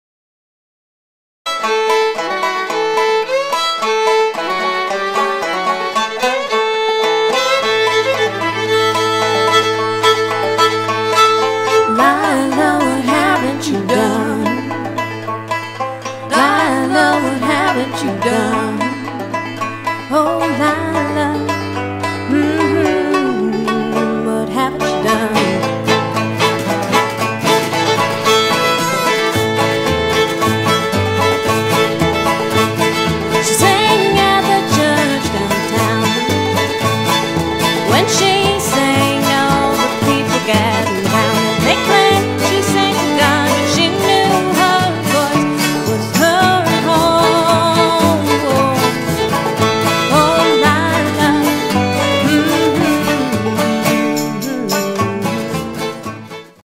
ittybittyplowplanecommittee_0001.wmv
the use of a petite record plow plane making two grooves in a board to be used in the creation of a small decorated receptacle to house the cremated remains of a dearly beloved feline companion.  all done to the sounds of the awful purdies musical group.